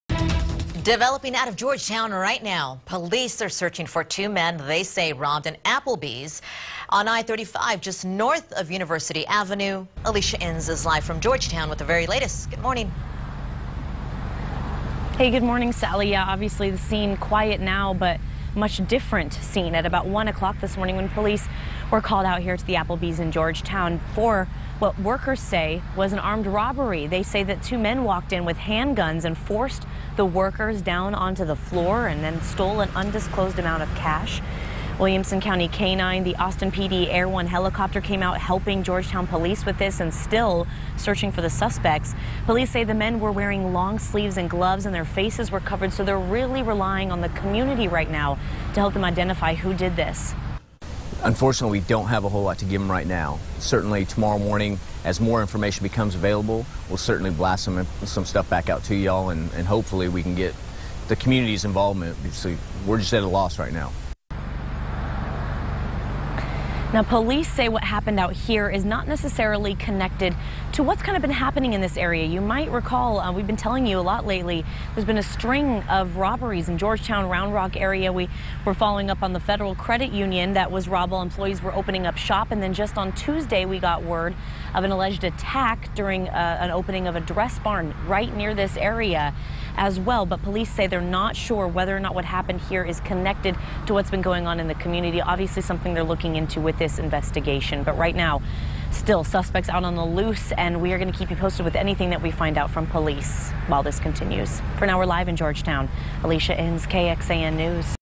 Georgetown Applebees robbed
Police searching for two suspects in an armed robbery